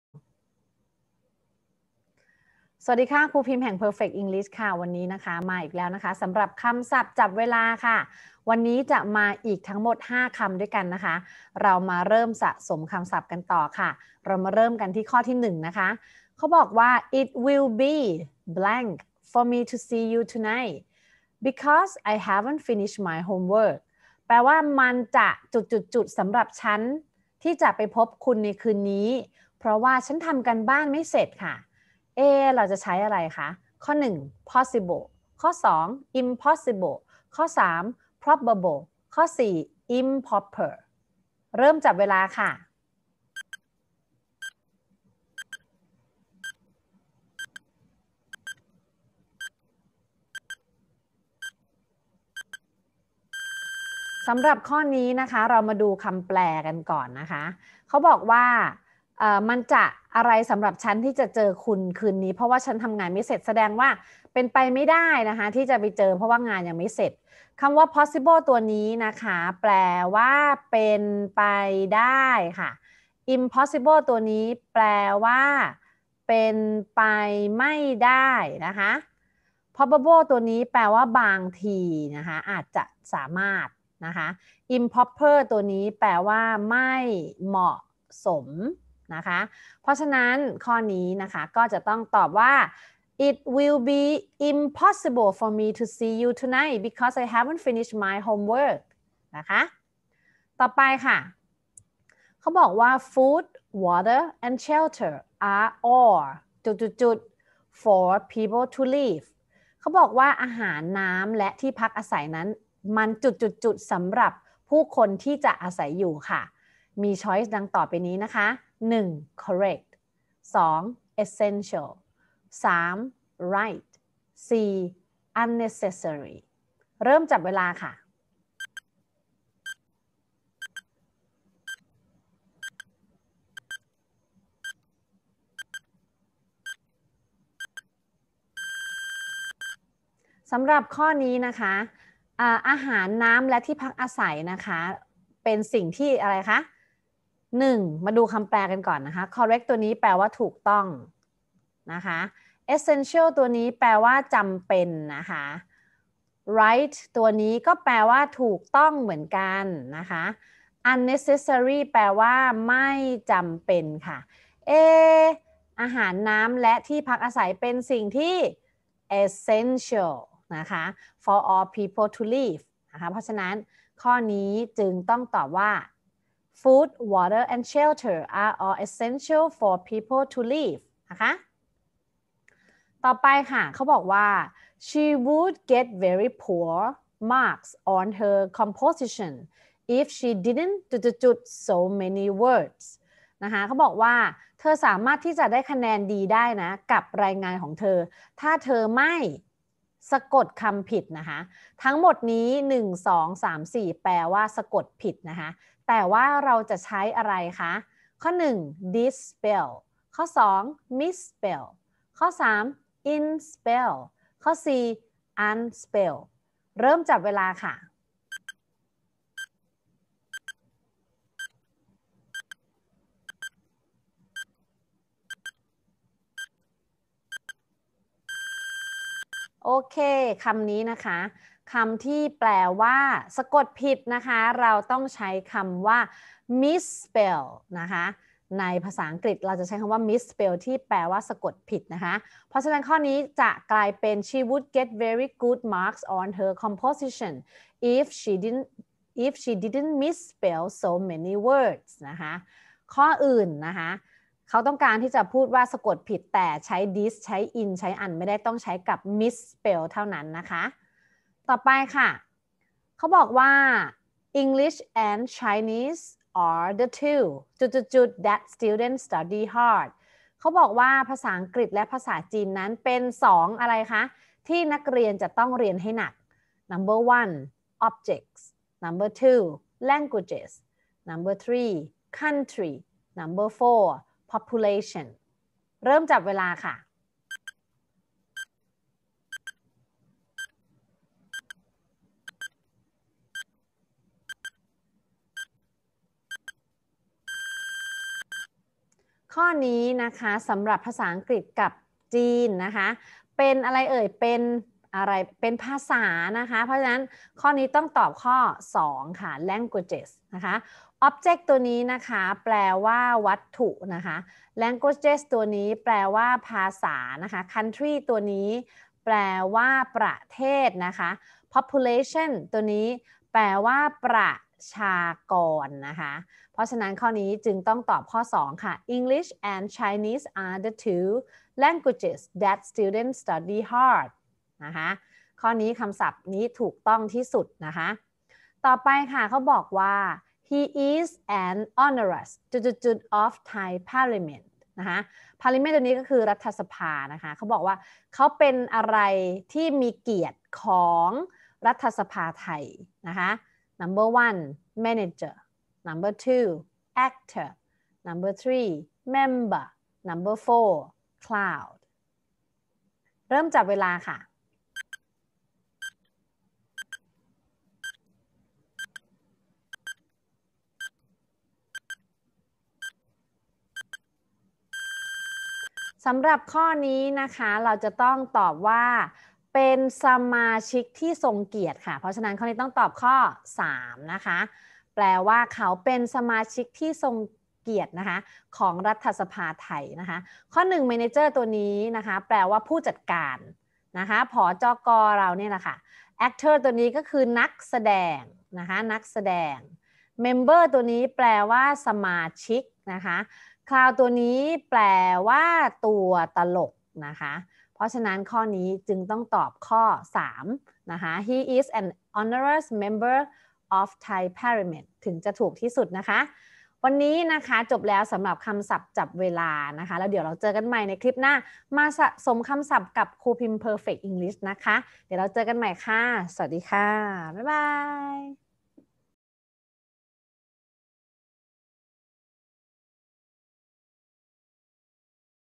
Perfect English l คำศัพท์จับเวลา (Part 3)
เพิ่มคำศัพท์วันละ 5 คำแบบจับเวลากันค่ะ